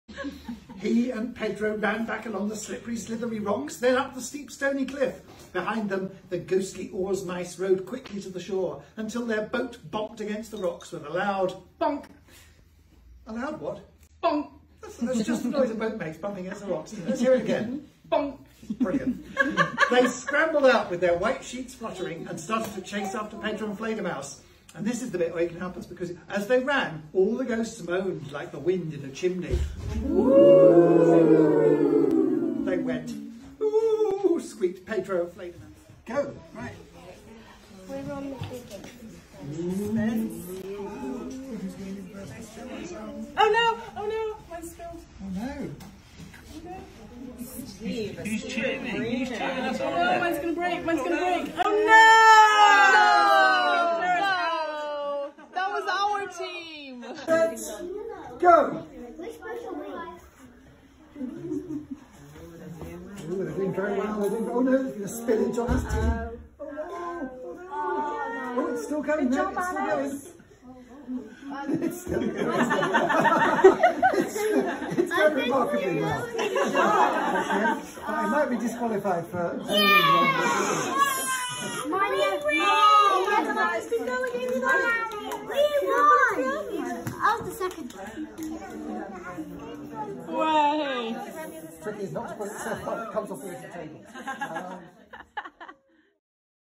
Adventuremice: The Ghostly Galleon launch party
Big thanks to First Draft Books for hosting our launch party for the fifth book in our series, 'Adventuremice: The Ghostly Galleon'! Be sure to check out this top-notch independent bookshop, inside Grace & Favour Home, next time you're in Bovey Tracey, Devon

Thanks to Cristina Guraliuc for some of this video footage!